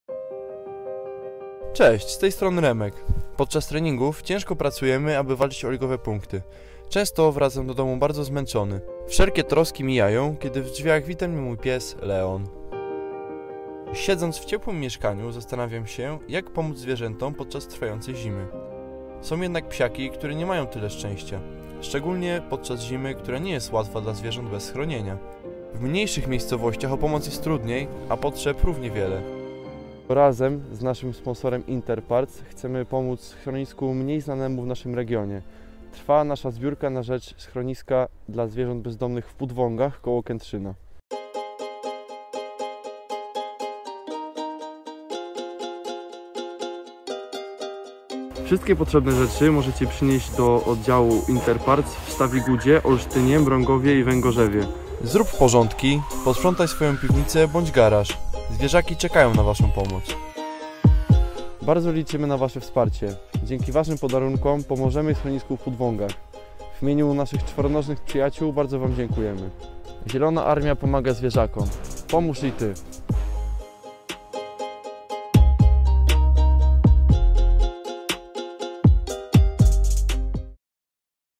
Remek Kapica pomaga zwierzakom!
Dla Remka spacery z jego czworonożnym przyjacielem Leonem, są odskocznią od życia codziennego. Nie każdy pies może jednak liczyć na ciepło rodzinnego domu. Zwłaszcza podczas trwającej zimy, która nie jest łatwa dla zwierząt bez schronienia. W mniejszych miejscowościach o pomoc jest trudniej, a potrzeb o wiele więcej.

Wspólnie z Inter Parts oraz zaprzyjaźnionymi gminami, przypominamy o naszej zbiórce dla naszych czworonożnych przyjaciół dla Schroniska dla Zwierząt Bezdomnych w Pudwągach k. Kętrzyna 💚

Zimowa zbiórka na rzecz schroniska w Pudwągach trwa całą zimę. Zróbcie porządki, posprzątajcie garaże. Zwierzaki czekają na Waszą pomoc 🐶🐱

Wszystkie potrzebne rzeczy możecie dostarczyć do oddziałów Inter Parts:
• Inter Parts Olsztyn - ul. Augustowska 17
• Inter Parts Stawiguda – ul. Jarzębinowa 4
• Inter Parts Mrągowo – ul. Olsztyńska 13
• Inter Parts Węgorzewo – ul. 3 Maja 15D

Zielona Armia pomaga zwierzakom – pomóż i Ty! 💚